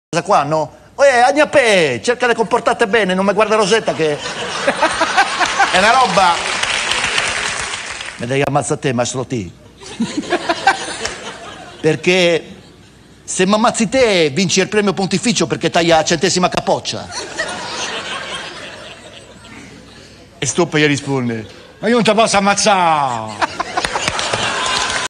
Max Tortora imita Celentano che parla in romano
Video tratto da Max Tortora imita Celentano in Rugantino e Paolo Stoppa